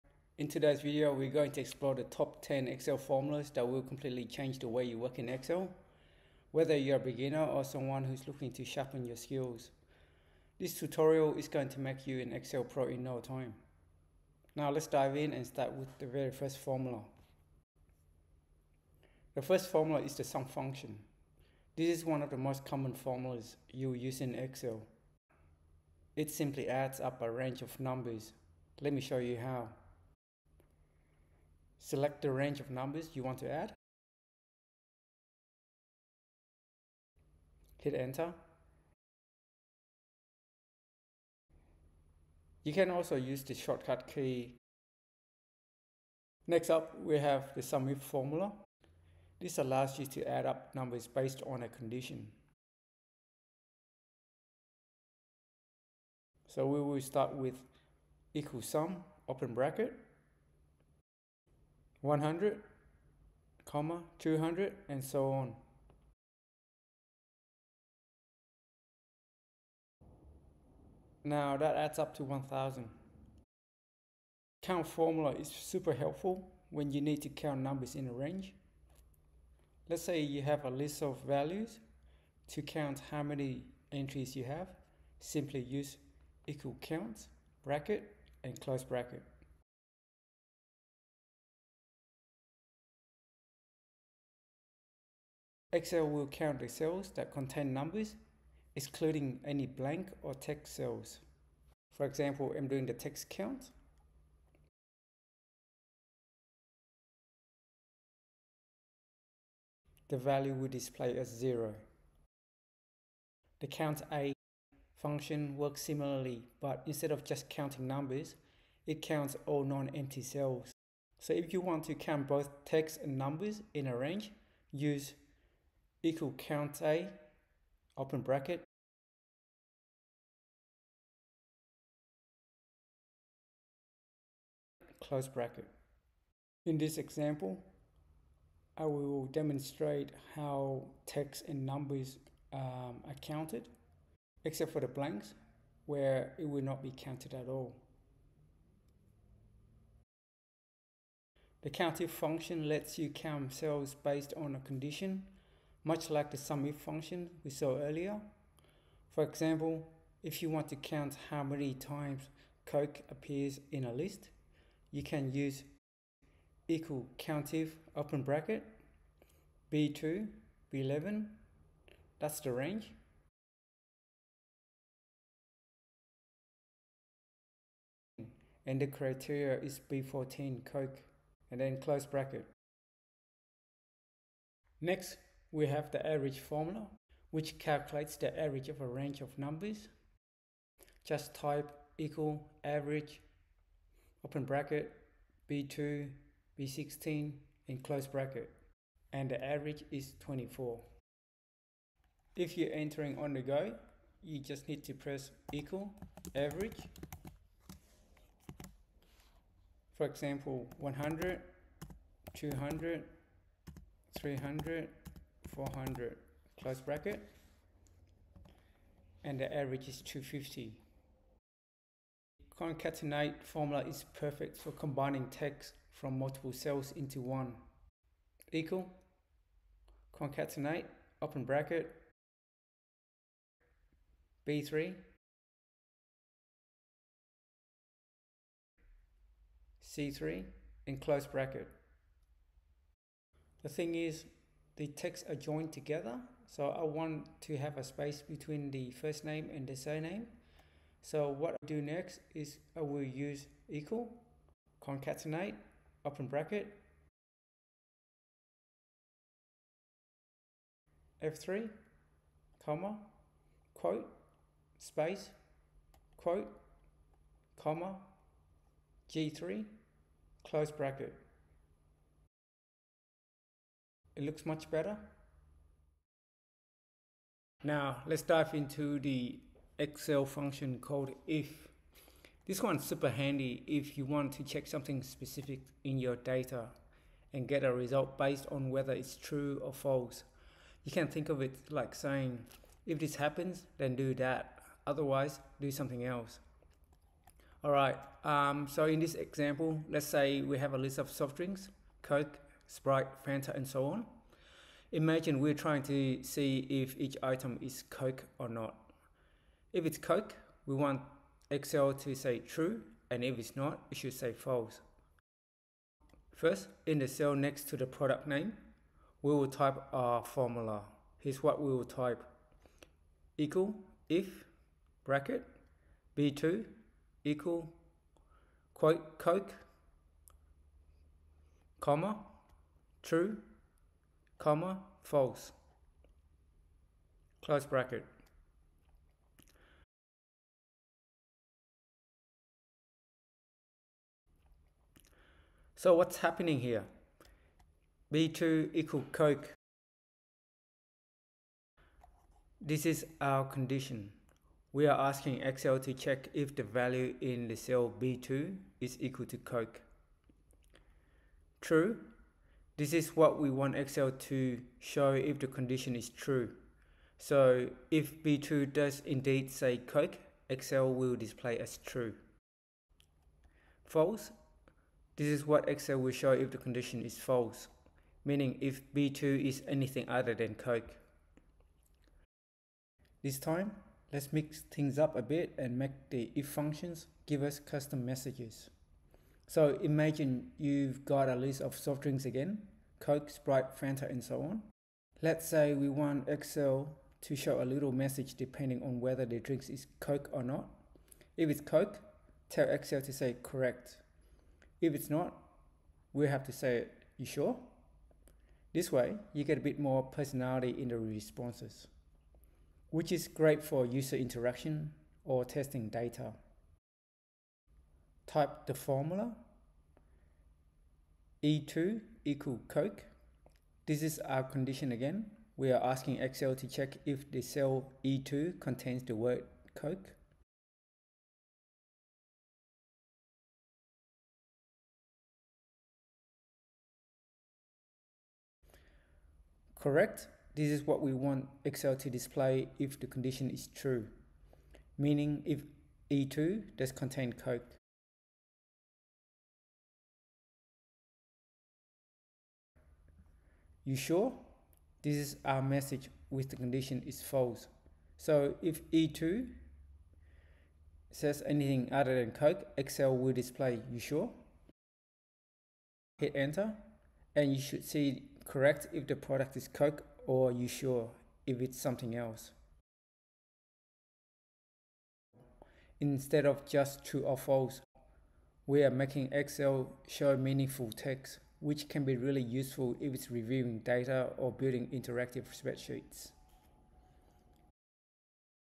8 Essential Excel Functions for Every Beginner
Master the basics of Excel with these 8 must-know functions! In this tutorial, we'll cover SUM, AVERAGE, SUMIF, CONCATENATE, COUNT, COUNTA, and IF functions, showing you how each one can simplify your tasks and save time. Perfect for beginners and anyone looking to improve their Excel skills, this guide will walk you through real-life examples, so you can start using these functions in your own spreadsheets. Join me and become more efficient in Excel!